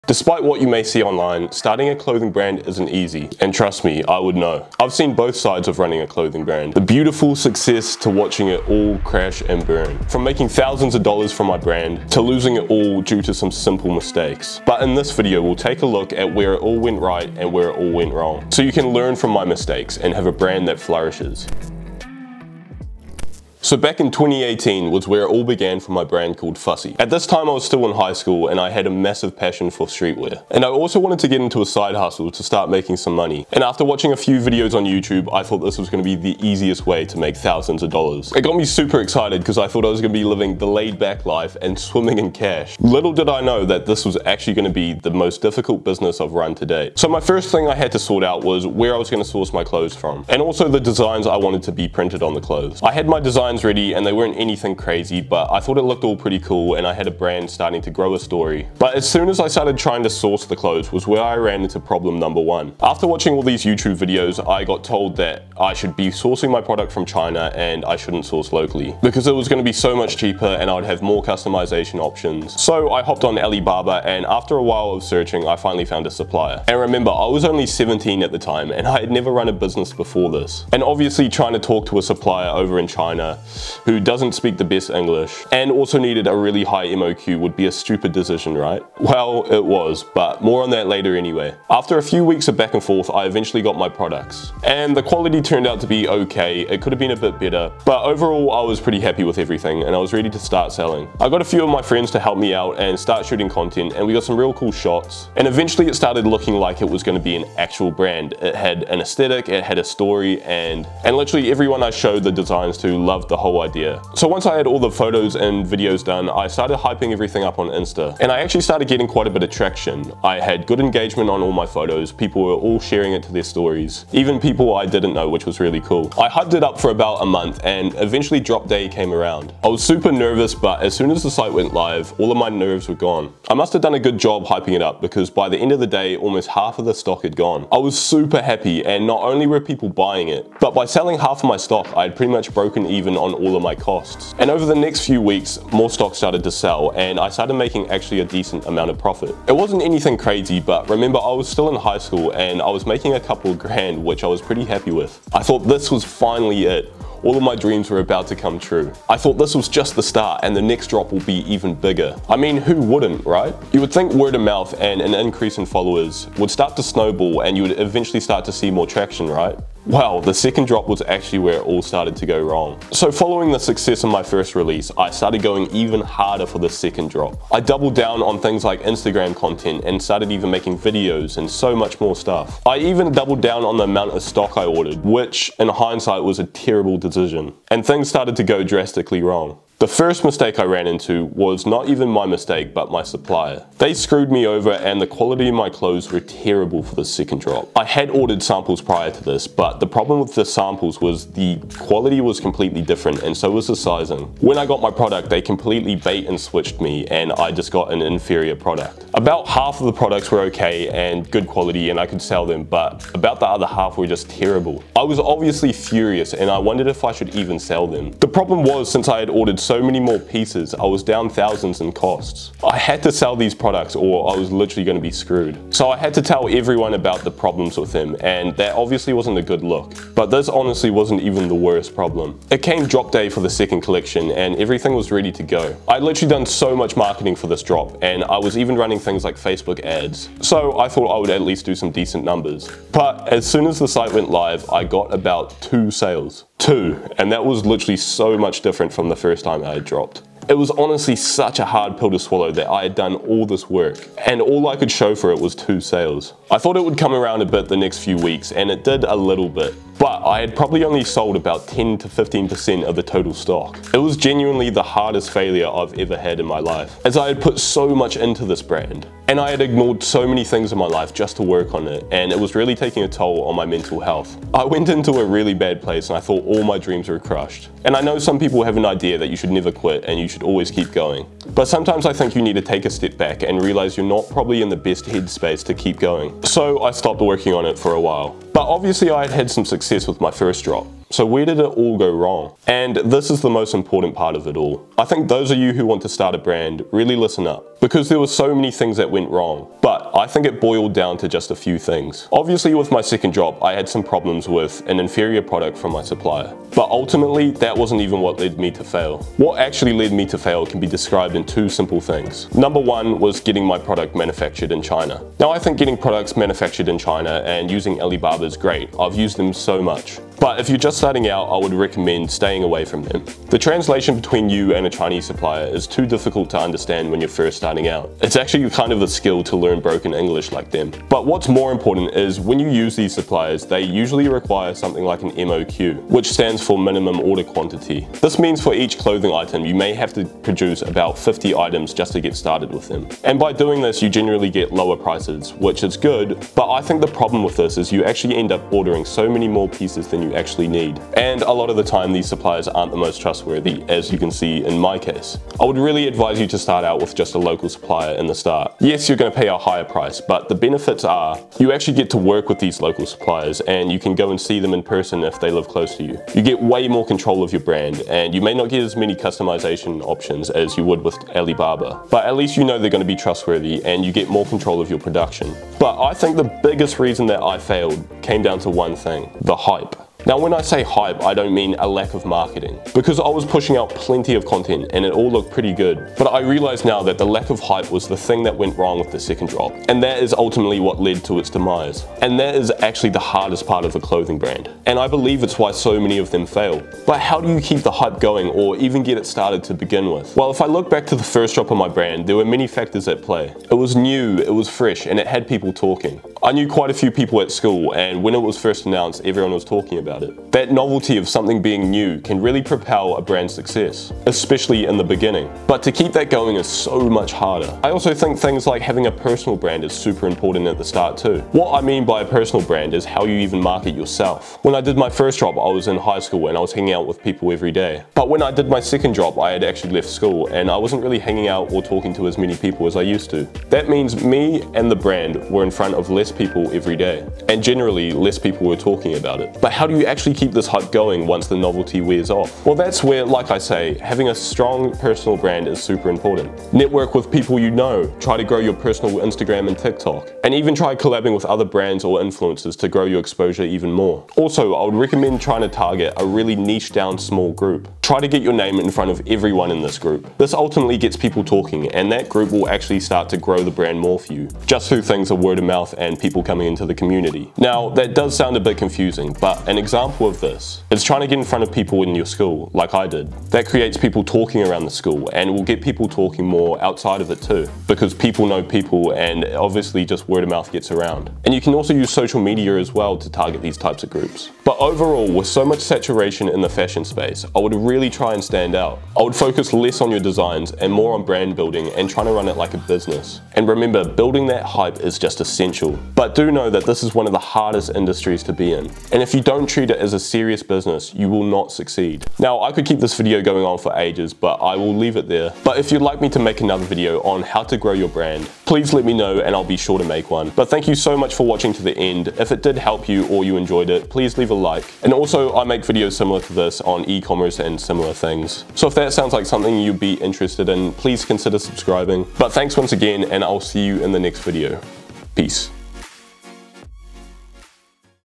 My Clothing Brand Failed and Here's What I Learned
I had great success with my clothing brand business - but it didn't last long. In this video we look at where it all went wrong and how you can run a successful brand to ensure you don't make the same mistakes as me.

⏲ Timestamps
00:00 — Intro
00:22 — The Start of My Brand
02:51 — Where It Went Wrong
04:51 — How You Can Succeed
08:06 — Conclusion